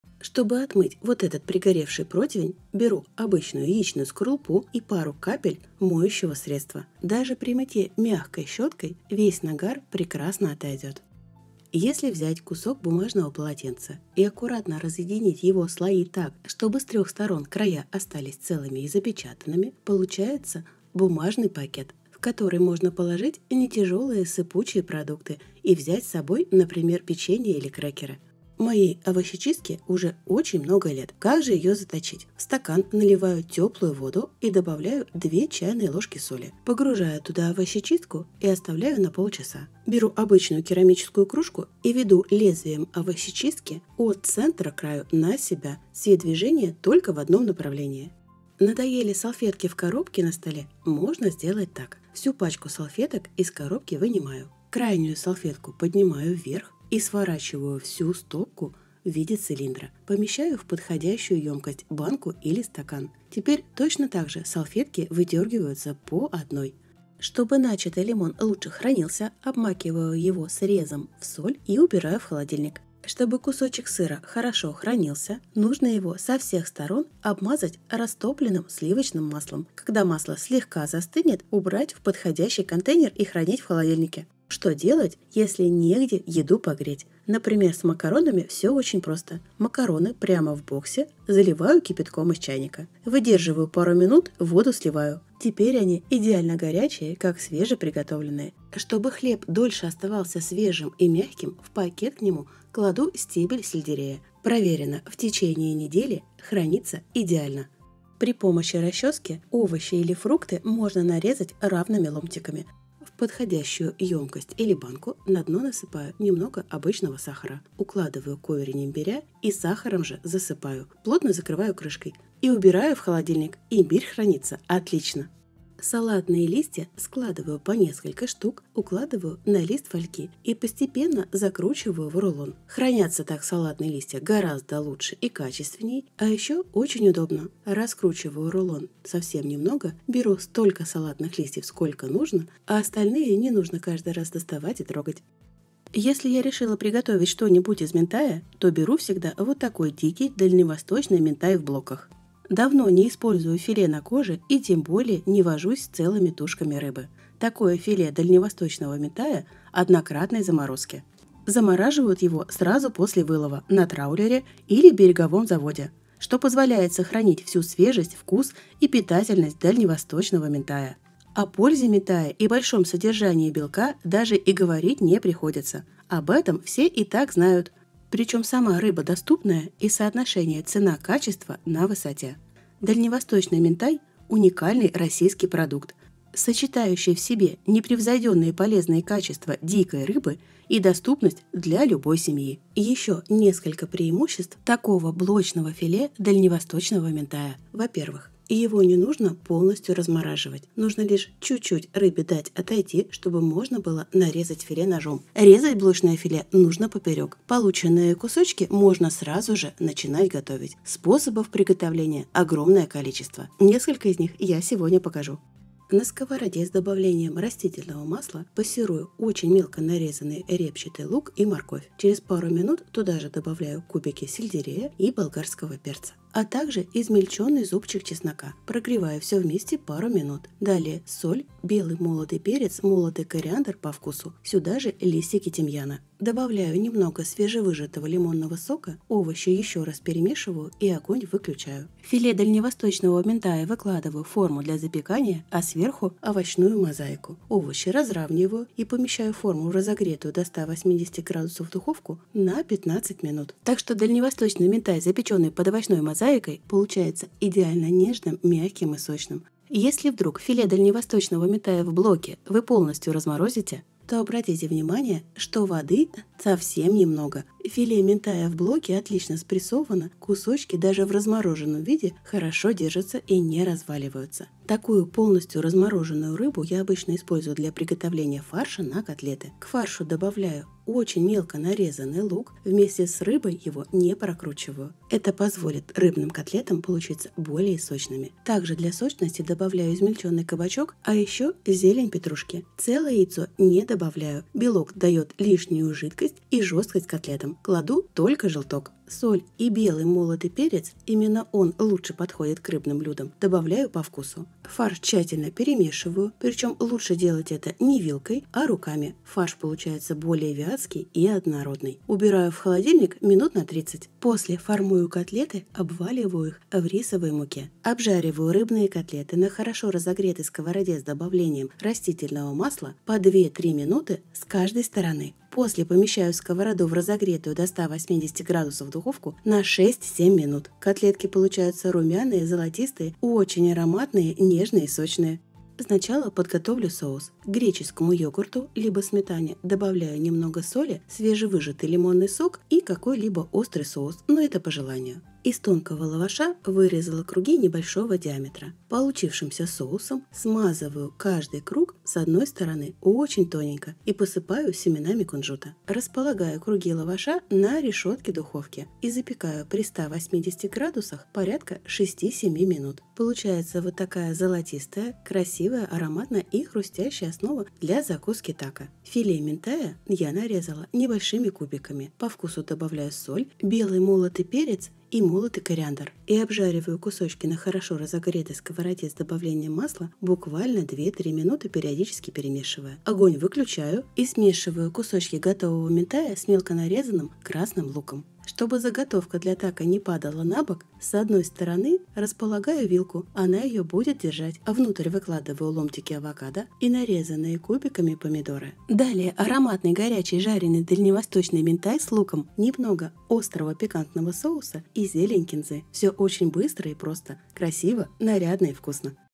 Жизнь научила! Советы и хитрости умных женщин
Тако с рыбой
Филе дальневосточного минтая 200 г
Лаваш тонкий 90 г
Авокадо 100 г
Помидоры 160 г
Репчатый лук красный 40 г
Сок лимона 10 г
Растительное масло 5 г
Греческий йогурт 2% 120 г
Кунжут 10 г
Острый соус Ширирача 5 г
Кинза 10 г
Соль
Перец белый молотый
На 100 г 128,3 кКал 10/4,9/11,6
Выход 632 г

Запечный минтай под овощной мозаикой
Филе дальневосточного минтая 400 г
Морковь 80 г
Перец болгарский 90 г
Репчатый лук 120 г
Стебель сельдерея 45 г
Тимьян
Кориандр молотый
Соль
Перец белый молотый
Петрушка
На 100 г 88,9 кКал 13,5/2,2/3,7
Выход 630 г

Рыбные котлеты сочные (без хлеба)
Репчатый лук 80 г
Кабачок 70 г
Петрушка 5 г
Желток яичный сырой 1 шт
Рисовая ц/з мука 25 г
Соль
Перец белый молотый
На 100 г 129,8 кКал 18,5/3,4/6,2
Выход 470 г

Реклама, НКО «Ассоциация добытчиков минтая»,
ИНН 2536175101
erid: LdtCKM2vX